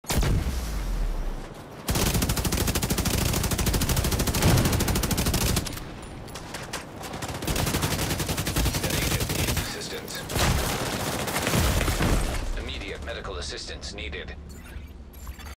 Dude freezes and nameplate dissapears
LEX Clan member freezes but this time gets caught